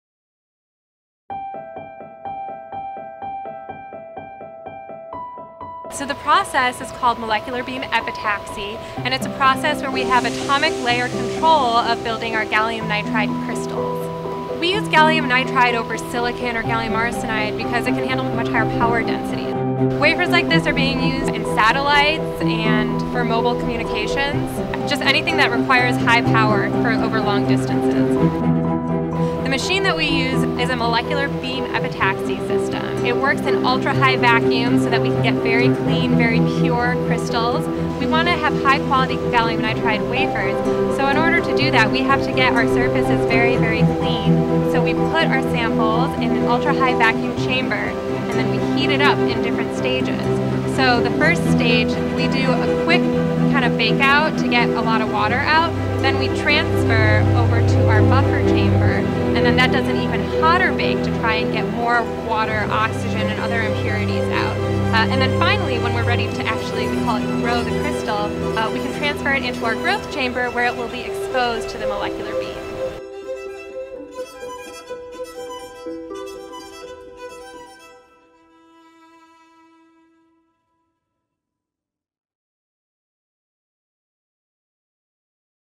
Gallium Nitride Loves High Power
Northrop Grumman’s Molecular Beam Epitaxy System synthesizes Gallium Nitride crystals for satellites, mobile communications, or anything that requires high power.